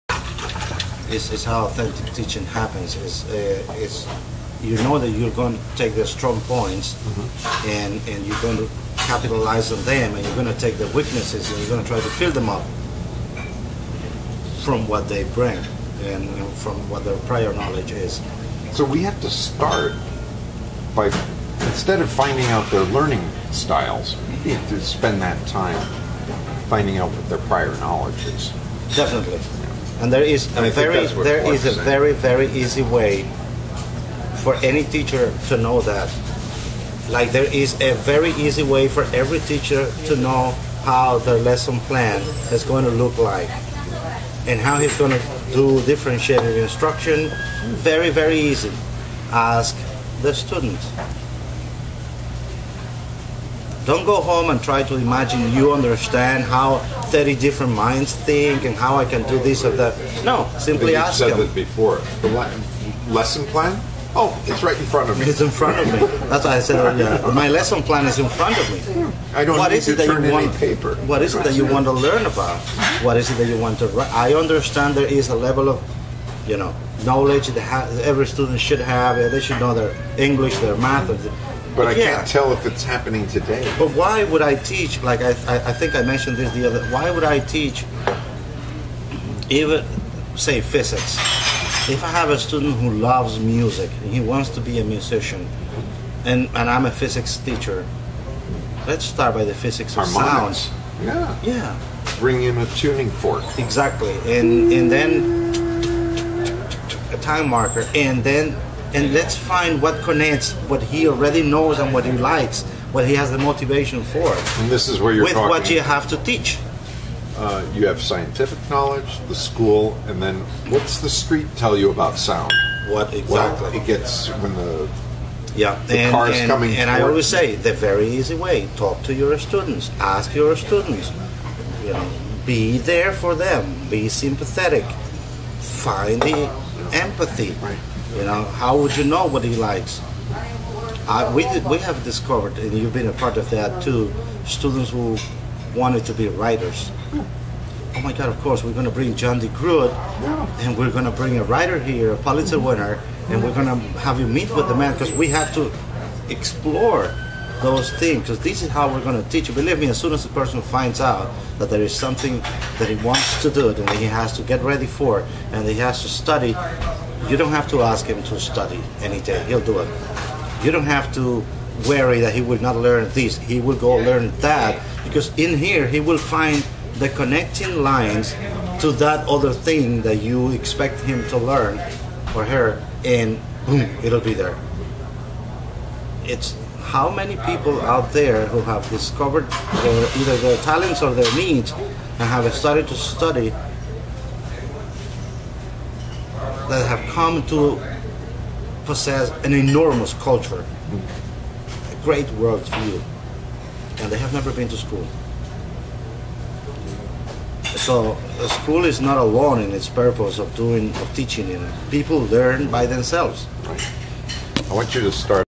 SANY0252.MP4 Mario talks about Education part 2 Clark and Prior Knowledge
Mario talks about Education part 2   Clark and Prior Knowledge

 "Mario Llorente" Education Clark "Prior Knowledge"